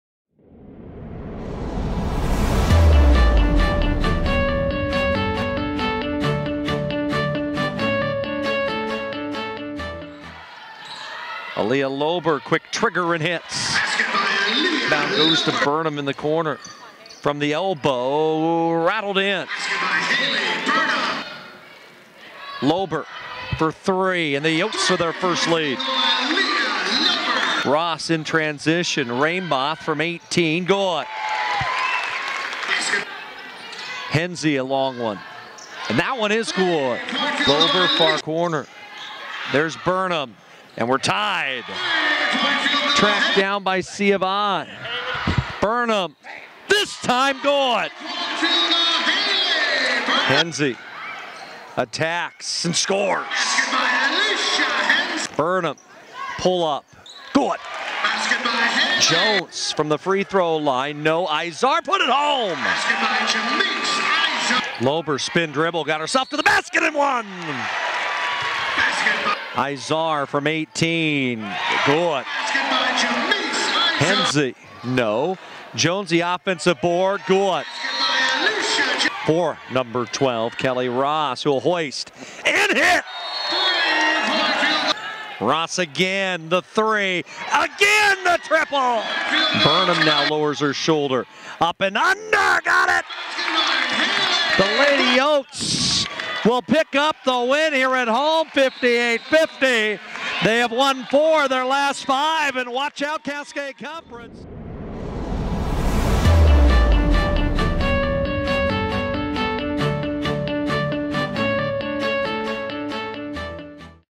College of Idaho Women's Basketball vs. Concordia (1/3/15)
Highlights of College of Idaho's 58-50 victory over Concordia University on Jan. 3, 2015